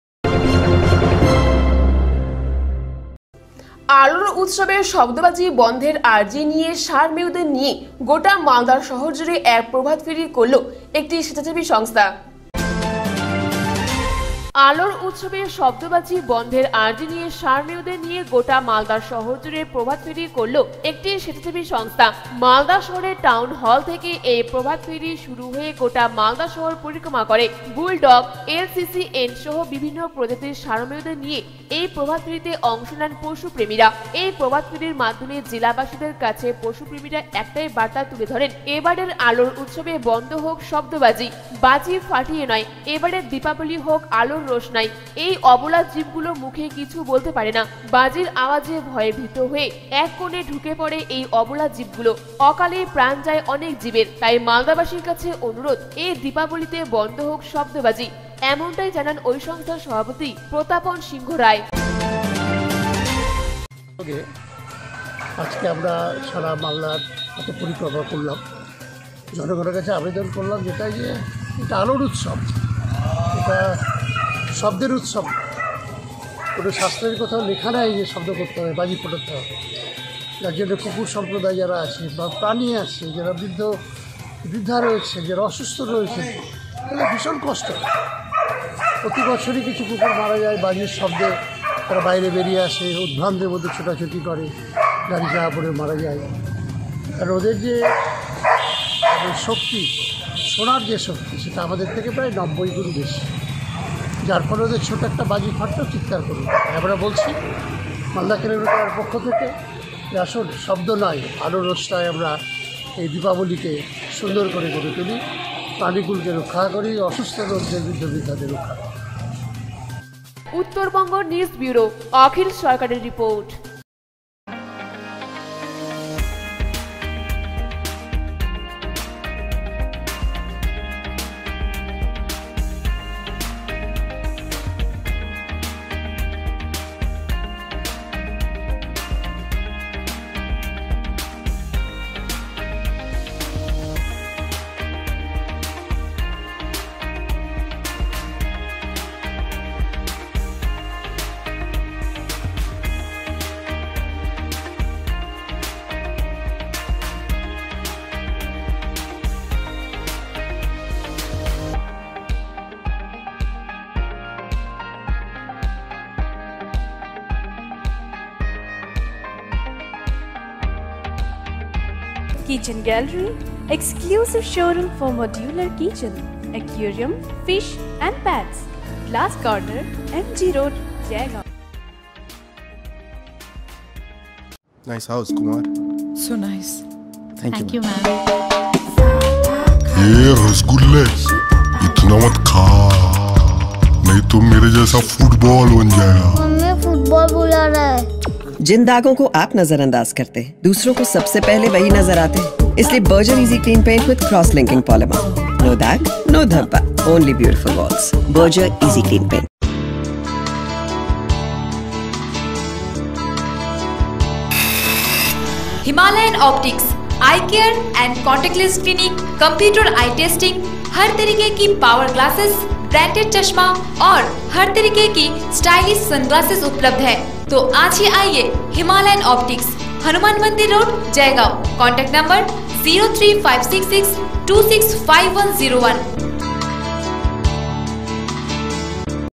আলোর উৎসবে শব্দবাজি বন্ধের আর্জি নিয়ে সারমেয়দের নিয়ে প্রভাতফেরী করল একটি স্বেচ্ছাসেবী সংস্থা
আলোর উৎসবে শব্দবাজি বন্ধের আর্জি নিয়ে সারমেয়দের নিয়ে গোটা মালদা শহর জুড়ে এক প্রভাতফেরী করল একটি স্বেচ্ছাসেবী সংস্থা